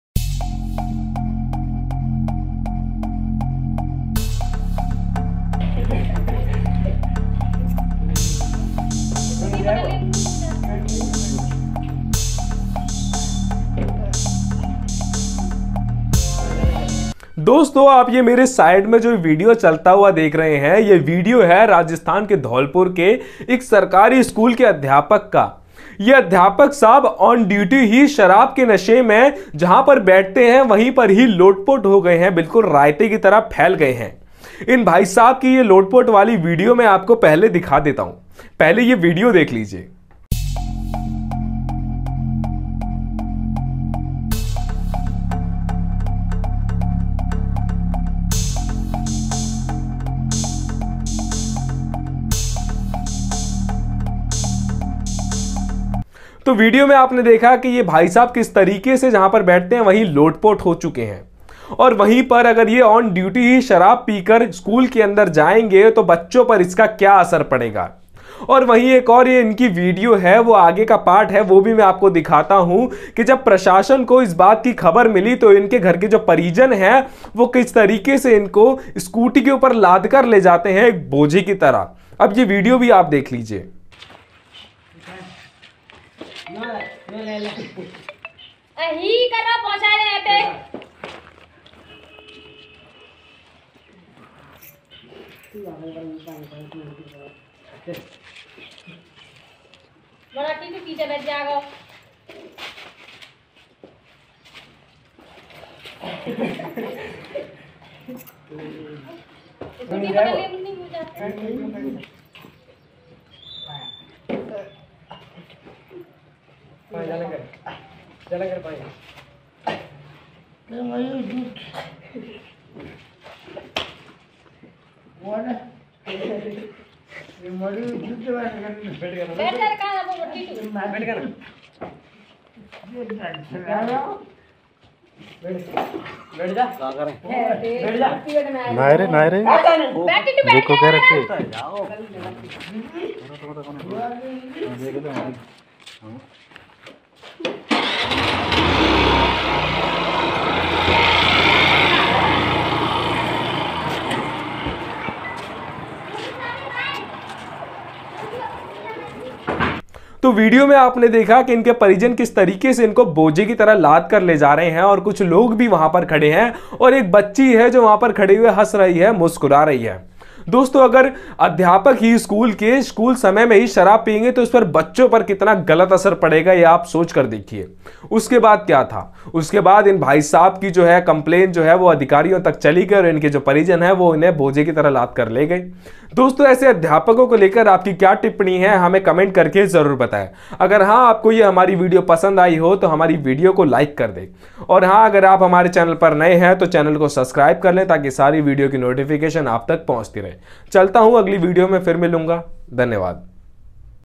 Dhaulpur teacher viral video टीचर दारू पीकर फुल टल्ली पंहुचा तो क्या हुआ | Teacher Stunt video
Dhaulpur teacher viral video स्कूल के बच्चों के सामने जब टीचर दारू पीकर फुल टल्ली पंहुचा तो क्या

The lollipops

SHOW YOUR PRECIOUS SUPPORT

Keep love